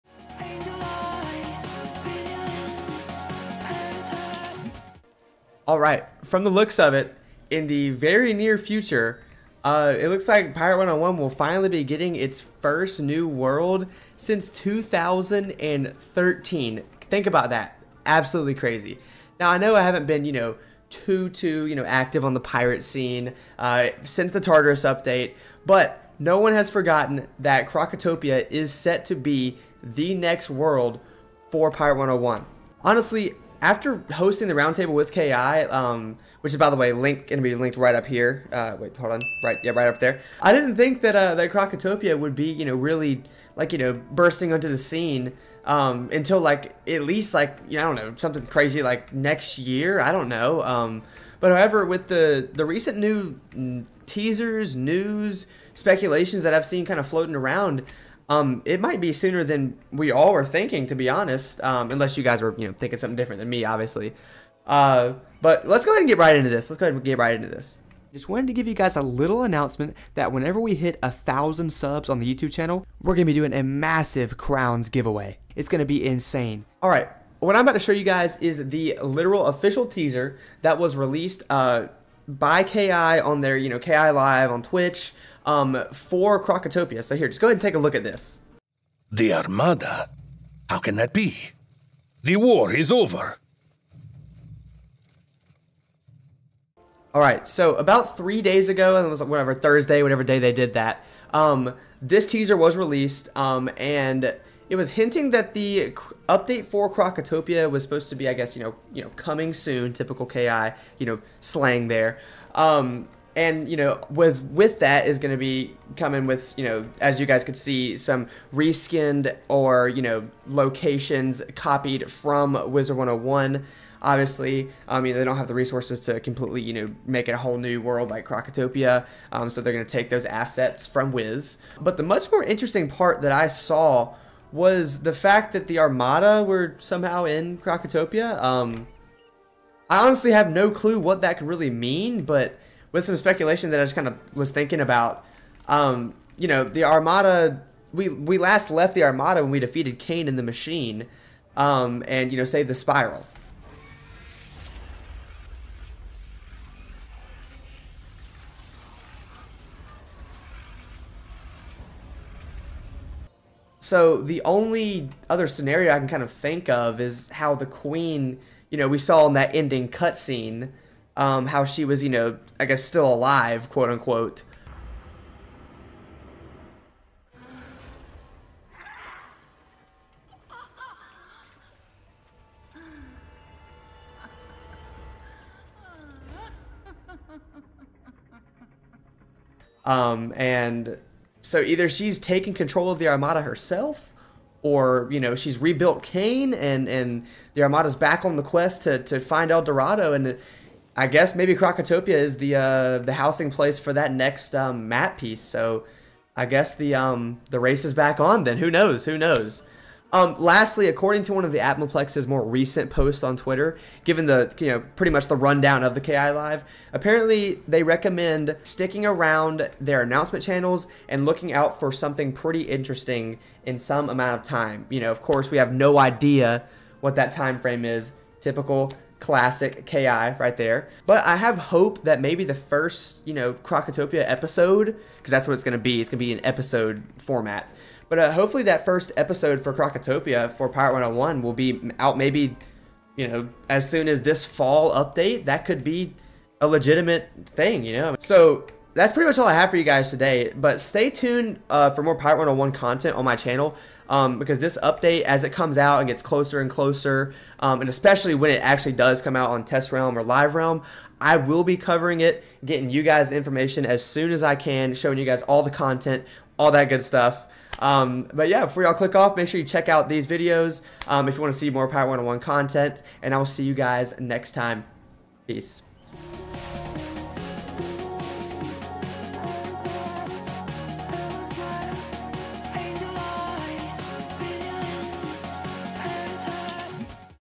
Pirate101 Just Revealed THIS About The Krokotopia Update!
Hope you guys enjoyed! Let me know what you think about the Armada coming to Krokotopia!